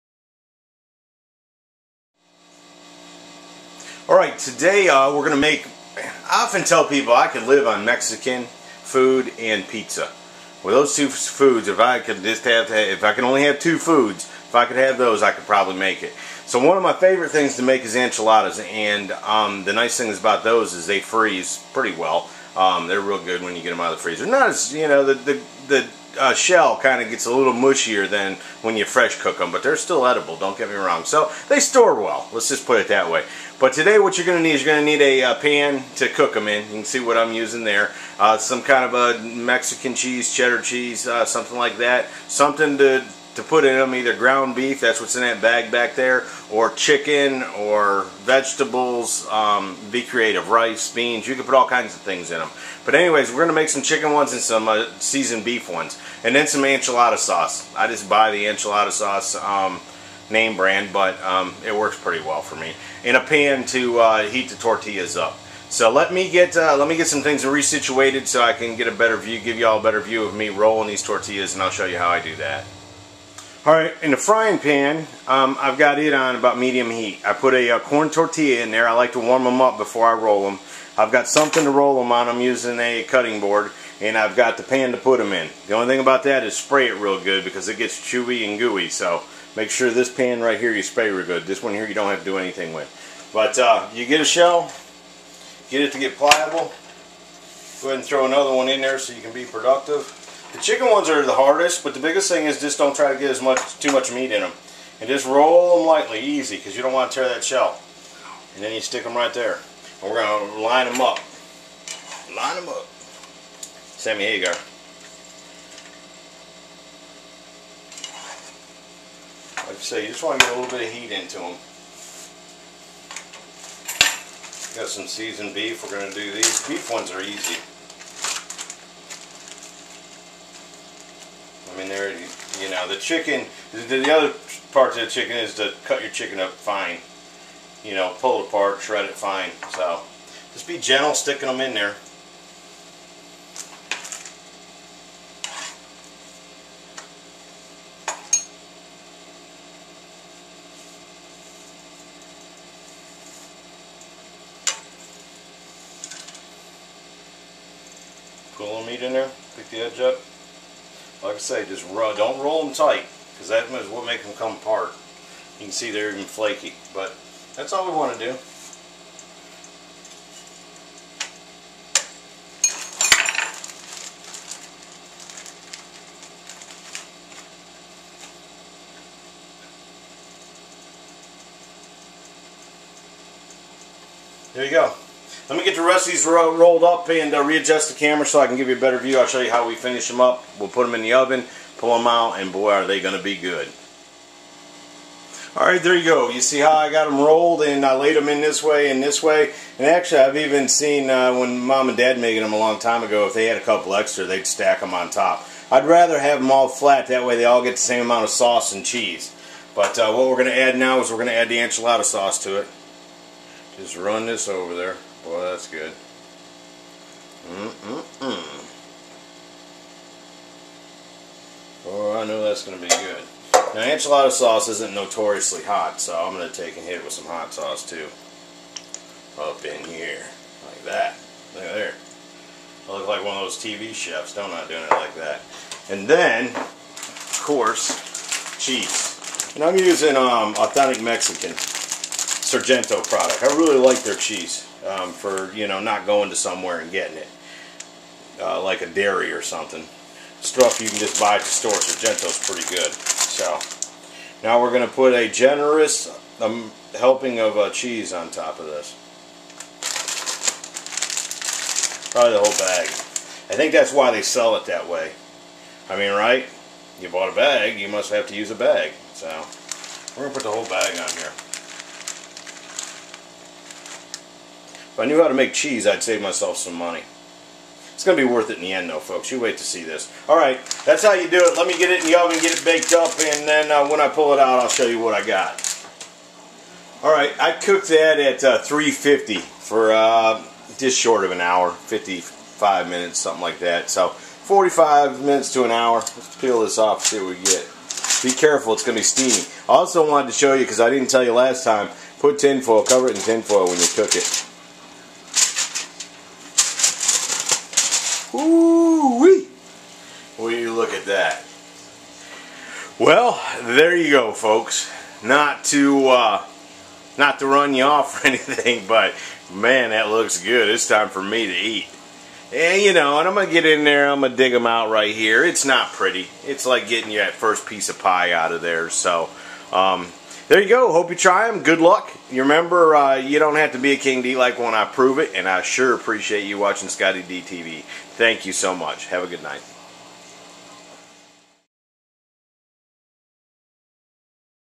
Beef and Chicken Enchiladas
I love Mexican food and this is one of my favorites...I show you how I make Beef and Chicken Enchiladas...they are very good...Enjoy!!